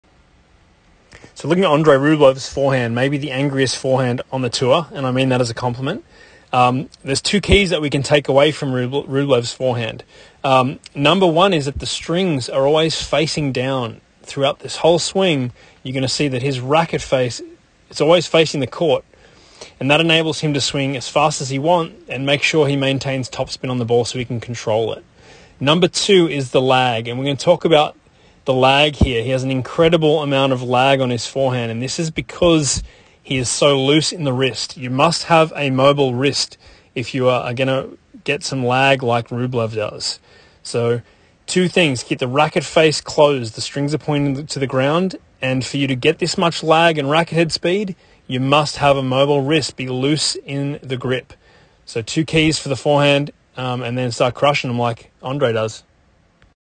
Andrey Rublev forehand keys
The Rublev forehand is a monster 🐉 ⠀
Two takeaways from this video 👆🏼⠀
1. Keep your strings facing down through the stroke⠀
2. “Lag” is created by a mobile wrist, it isn’t forced.⠀
Original Video Credit: 12kgp-tennis YouTube 👏🏼⠀